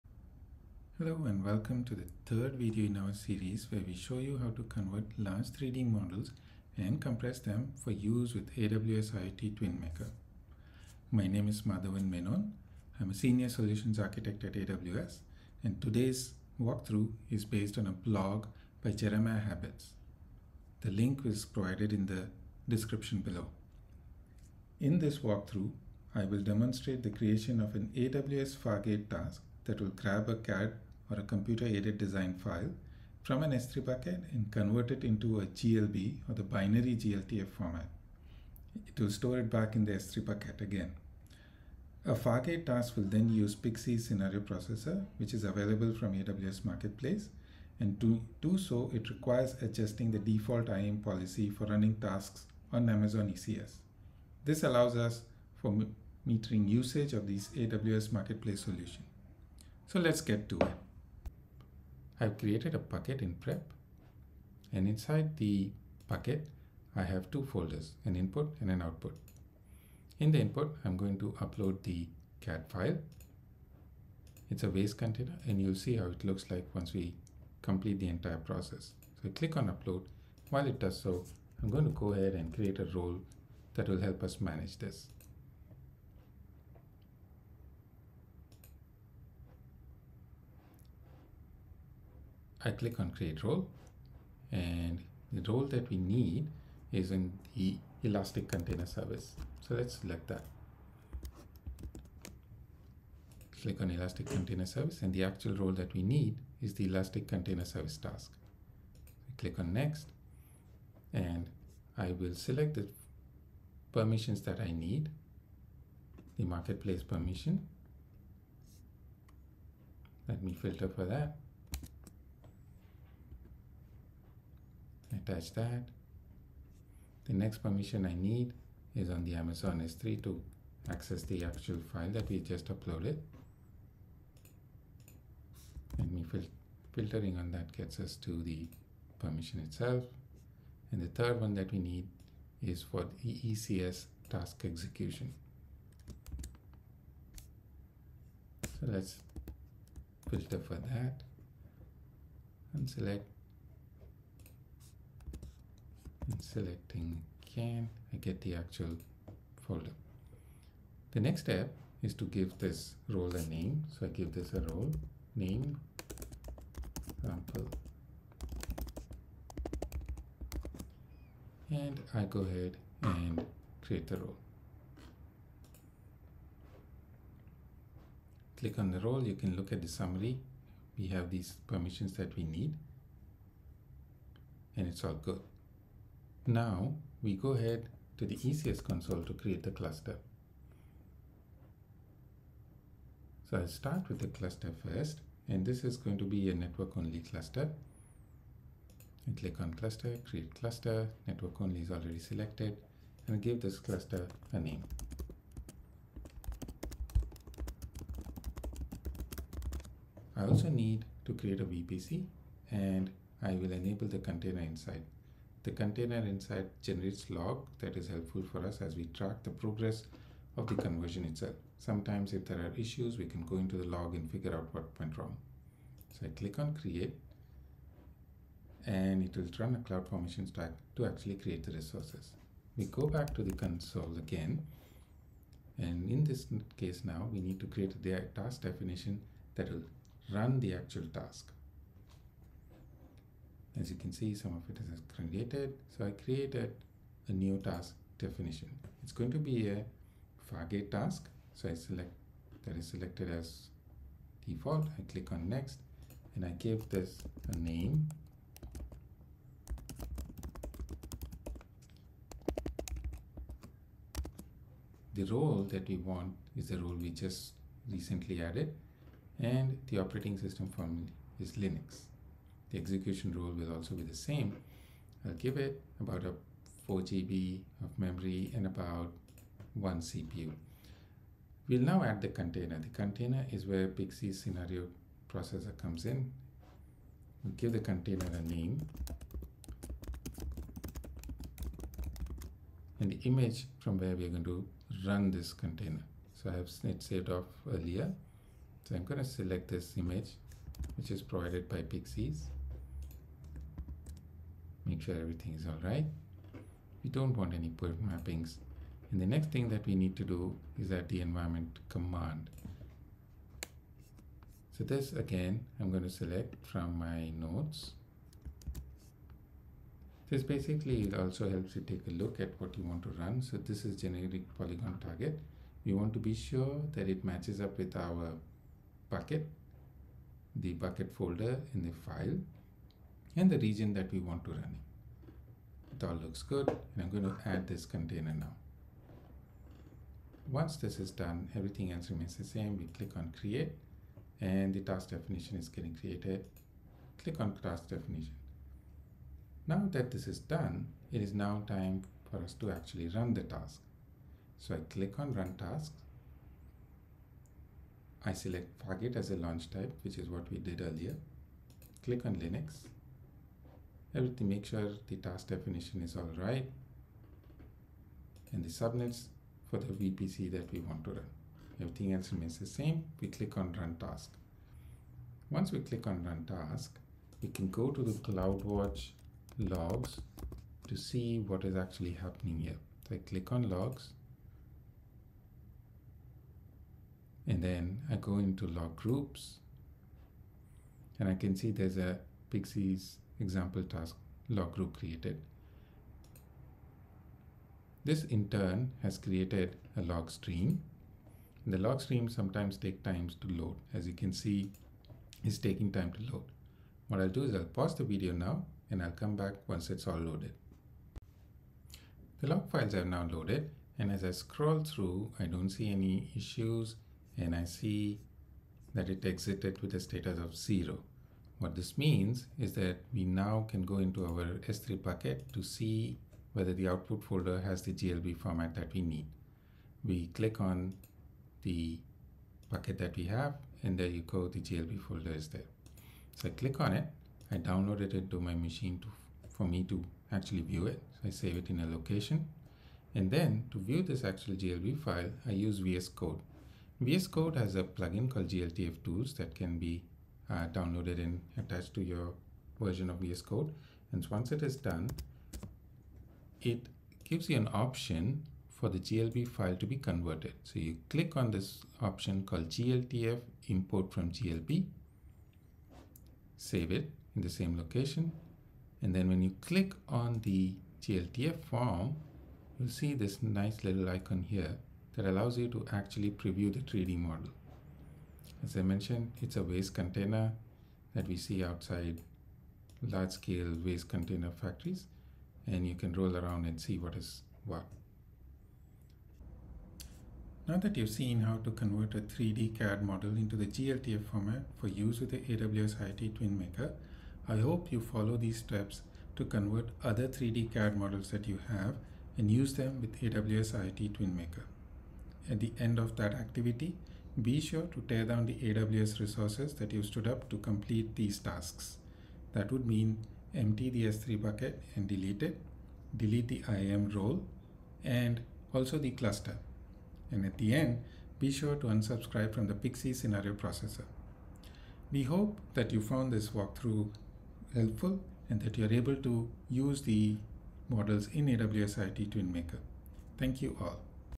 Convert and compress large 3D models for use with AWS IoT TwinMaker (CAD to glTF)
Description:
In this video we will run through Jeremiah Habet’s blog on how to convert and compress a large 3D CAD asset into the supported glb/gltf AWS IoT TwinMaker format. This is the third video in a series that will take you through the different tools and methods that allow for a smoother conversion/compression method of 3D models so you can get to building your digital twins faster.

Chapters:
00:00 Introduction
00:27 Architecture
01:02 Upload the 3D CAD Model
01:29 Create an IAM Role
03:06 Create Cluster
03:53 Create a Task Definition that goes with the cluster, and use the Pixyz Scenario Processor Container
05:50 Run Task Definition
06:52 View the glTF Formatted File
08:09 Recap

Links:
AWS IoT YouTube Channel
How to convert and compress 3D Assets into glb/glTF;
Pixyz Scenario Processor